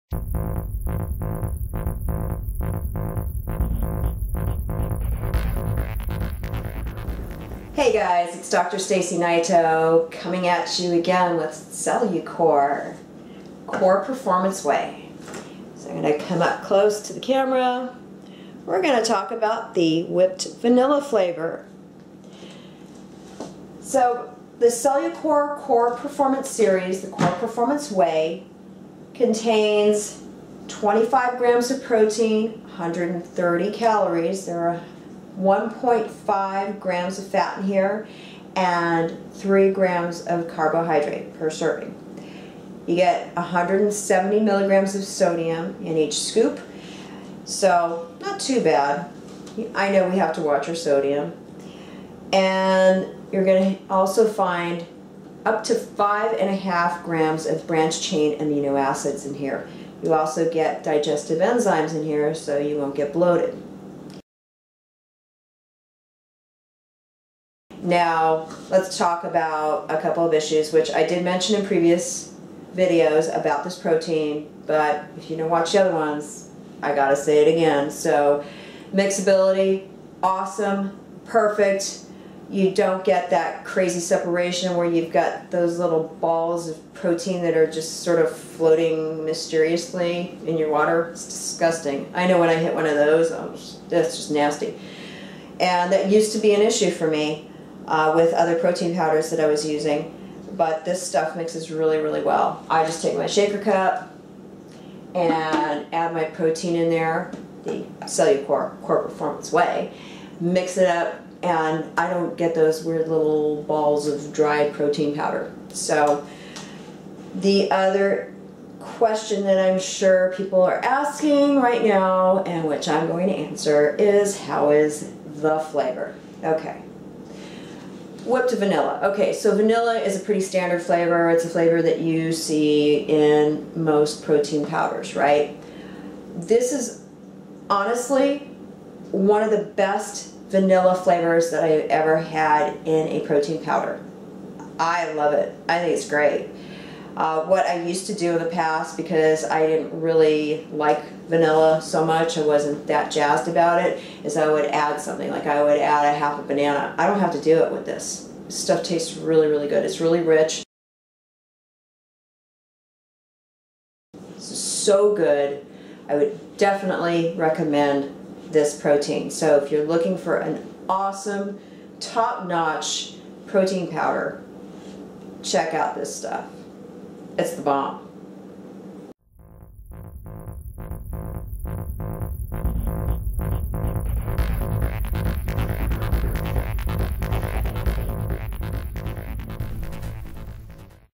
Cellucor Whipped Vanilla
Here is my review of the Whipped Vanilla flavor of the Cellucor Cor-Performance Whey...great stuff!